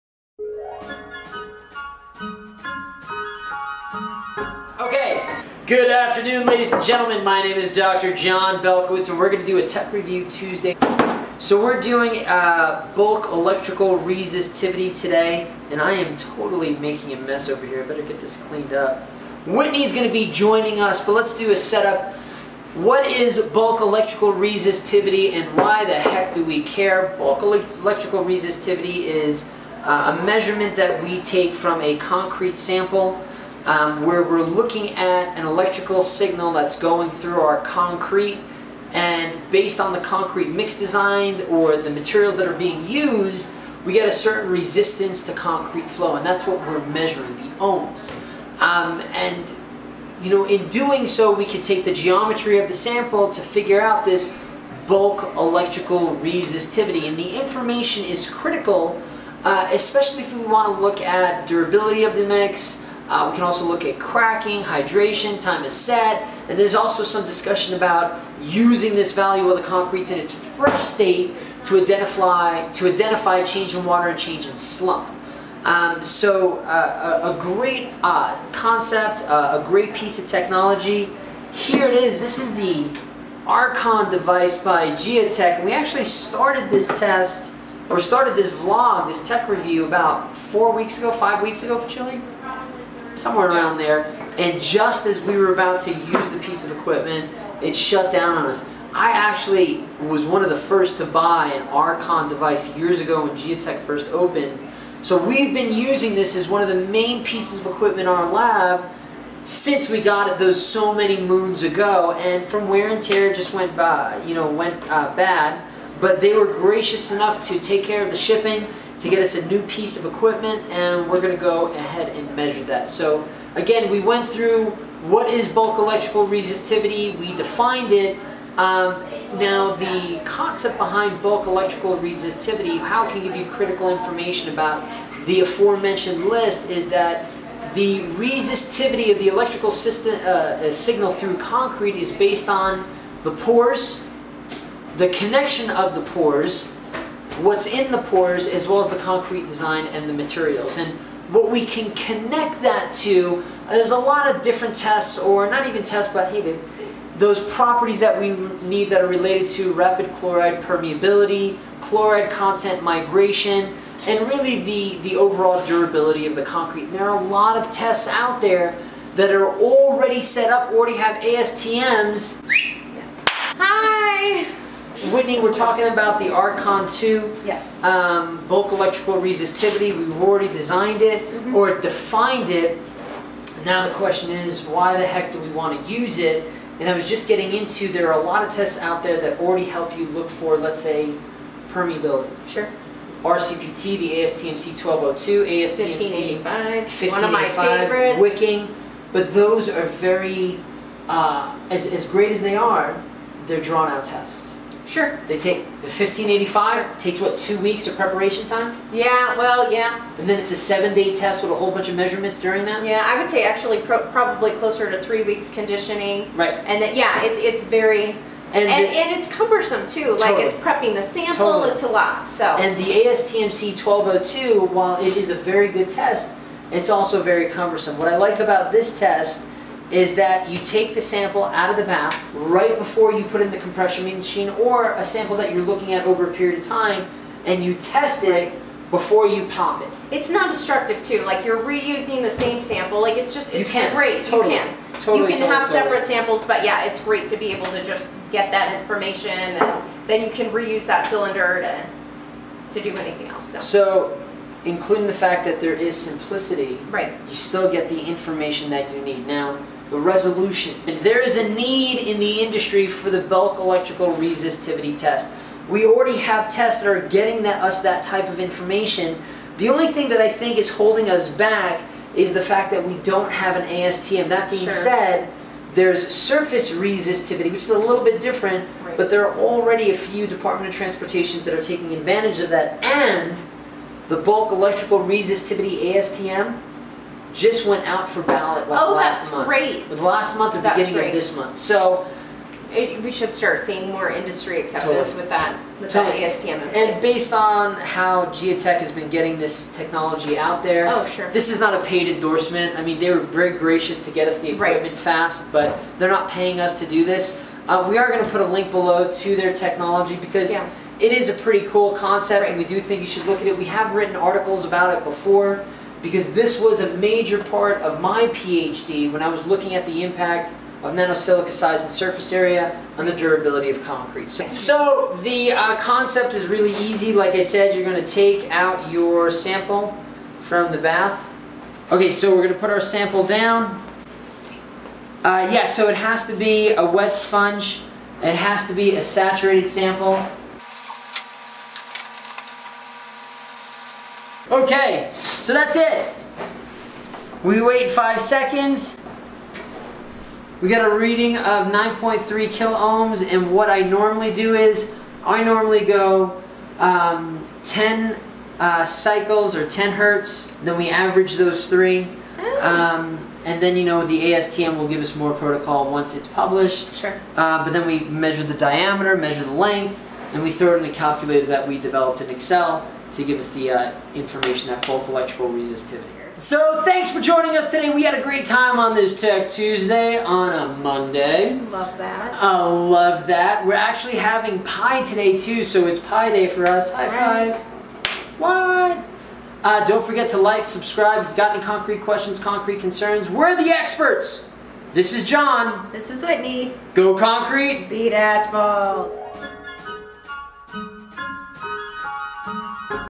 Bulk Electrical Resistivity Testing - Vlog #83
Dr. Jon is in the lab today discussing bulk electrical resistivity, and an awesome machine used to test for this resistivity.